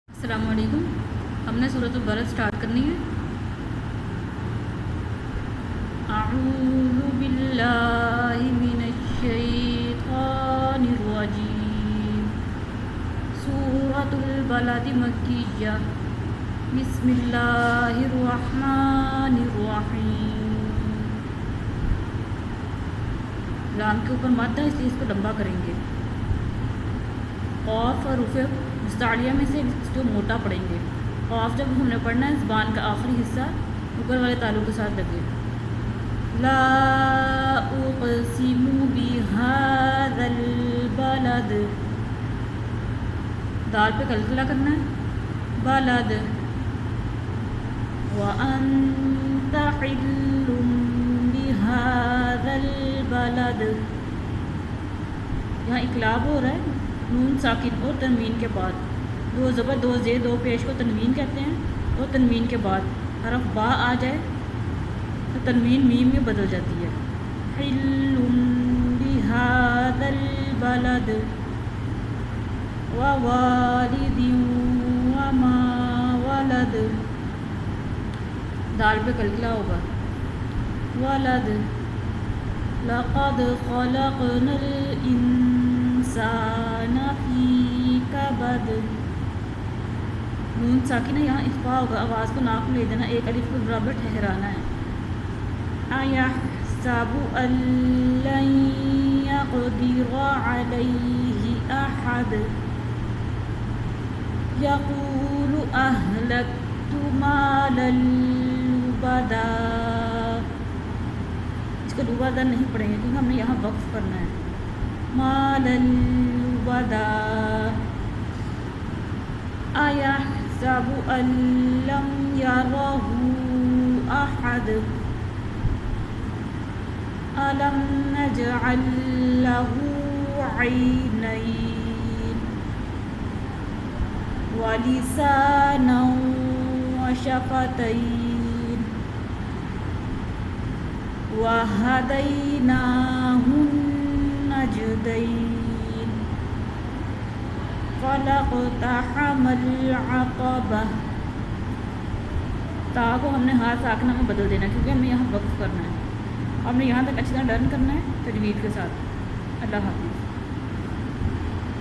Class 2 Para no 30 surat albald (line no 1to 11) learn with tajweed
Video from Miss Rozina